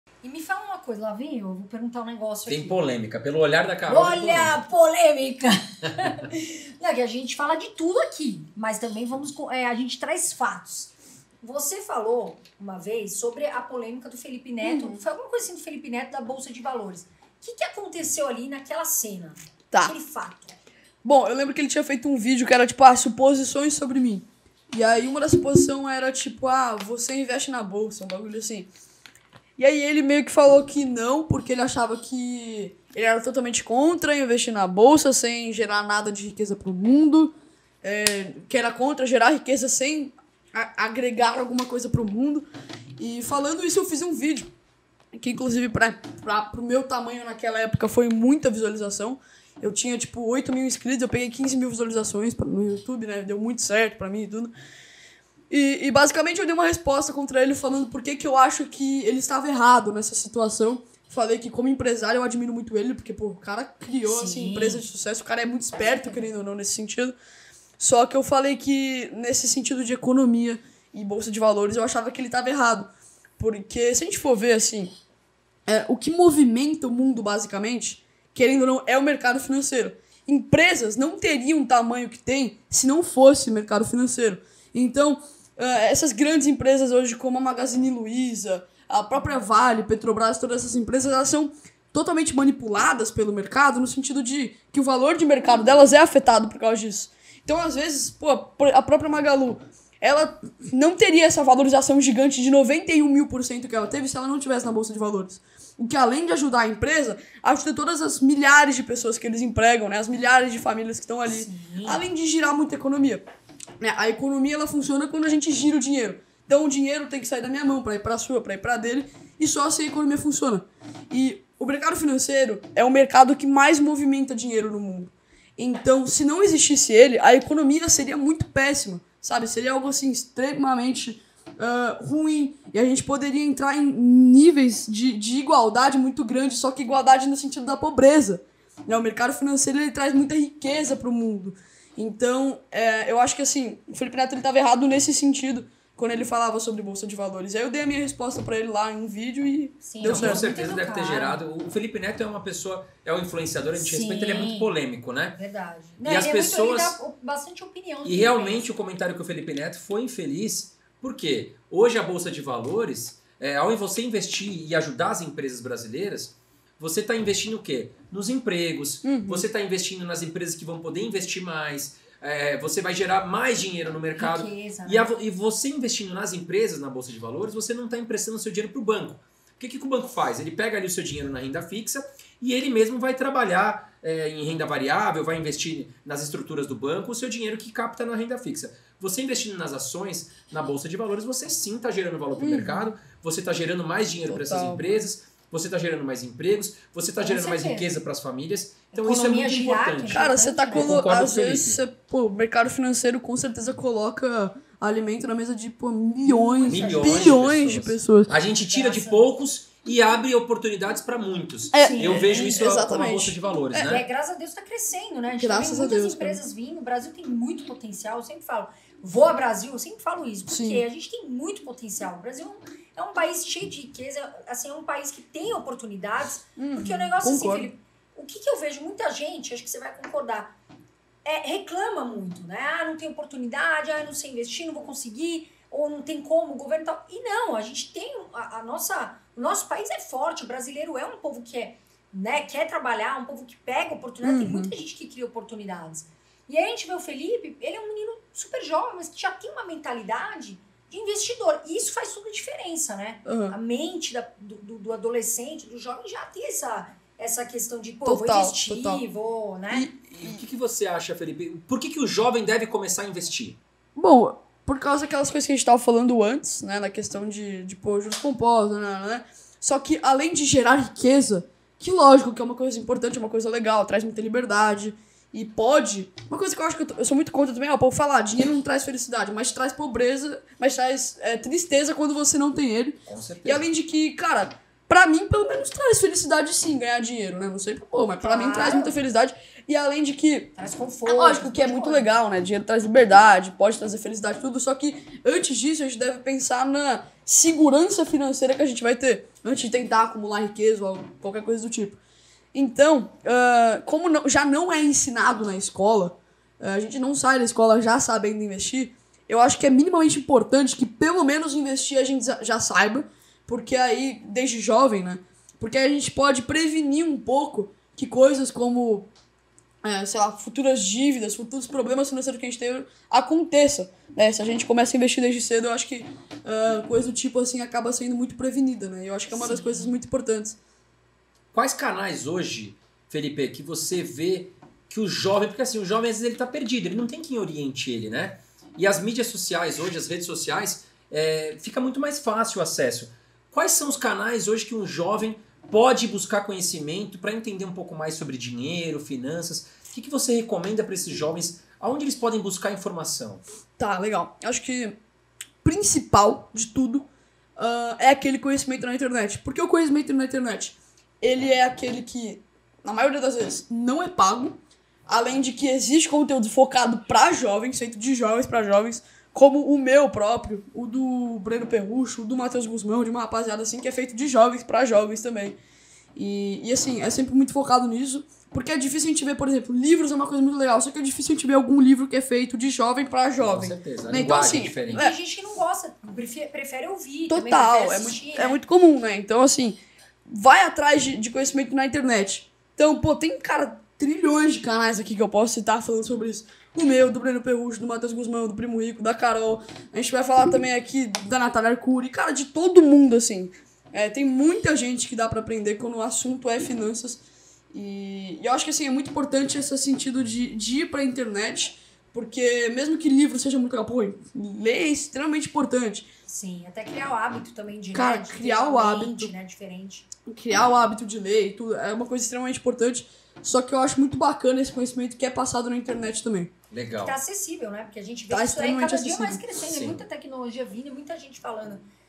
FELIPE NETO ERROU quando falou sobre a BOLSA DE VALORES | Cortes Irmãos Dias Podcast #3
FELIPE NETO ERROU quando falou sobre a BOLSA DE VALORES | Irmãos Dias Podcast  #3

Segundo Felipe Neto "BOLSA DE  VALORES NÃO GERA RIQUEZA"
Será? Qual a sua opinião sobre isso?
Vem saber qual foi a opinião dos Irmãos Dias e Felipe Molero.

Felipe Molero (@ofelipemolero)